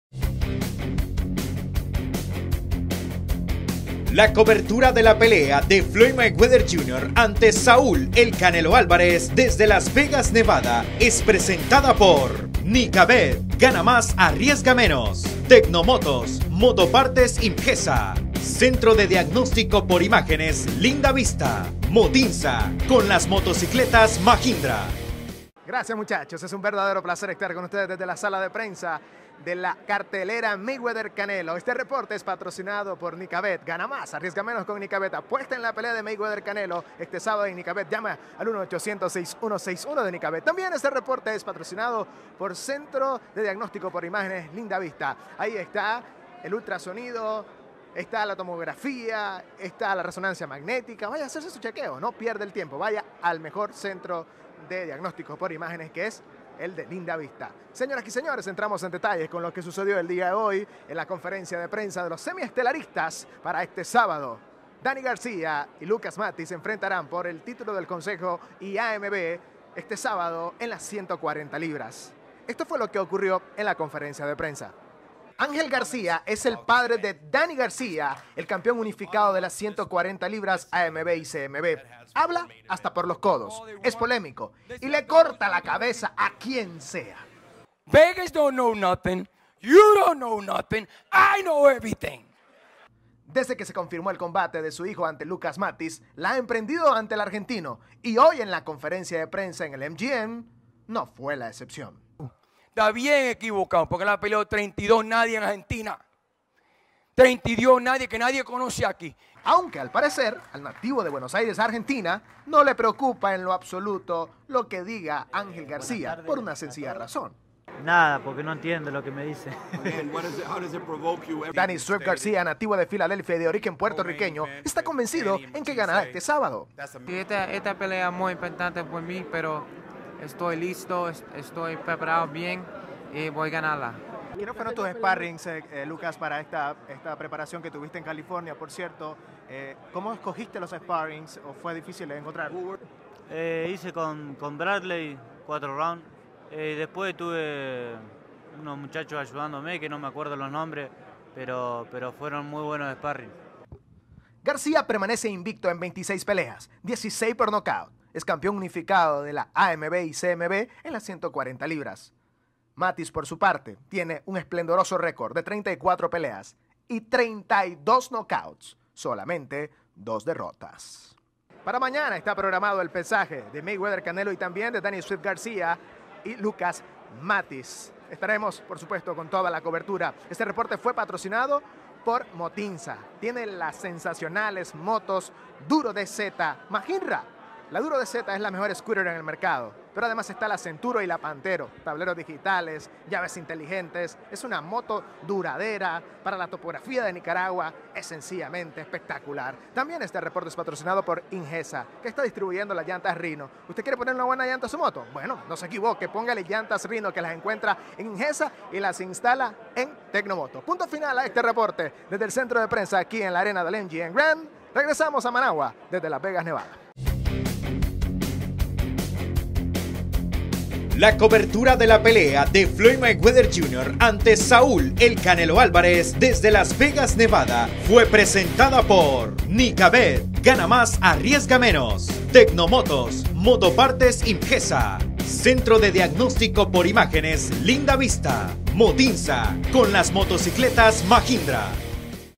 LLENOS DE DINAMITA
.EL PADRE DE DANNY GARCÍA LA EMPRENDIÓ EN CONTRA DE LUCAS MATTHYSSE
.EL SWIFT ESTÁ SEGURO DEL TRIUNFO EN LA PELEA MÁS IMPORTANTE DE SU CARRERA